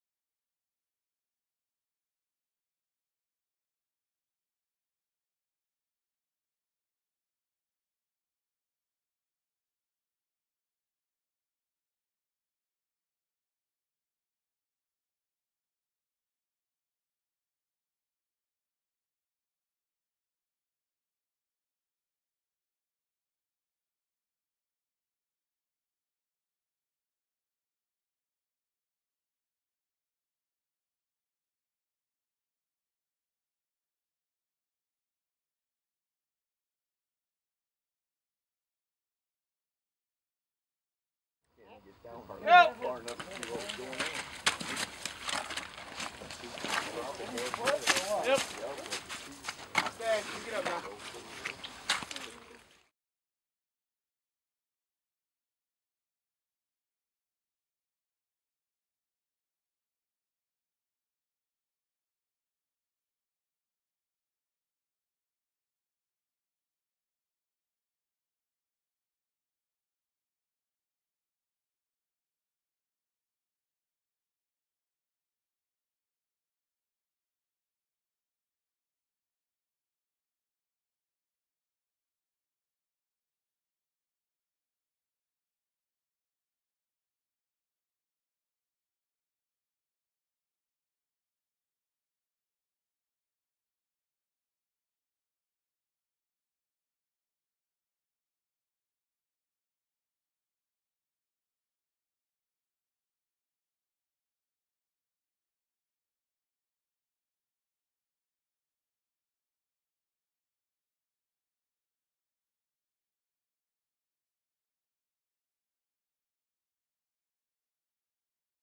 Plain Township Fire Department Ice Training
Plain Township Fire Department trained in ice rescue at Tippecanoe Lake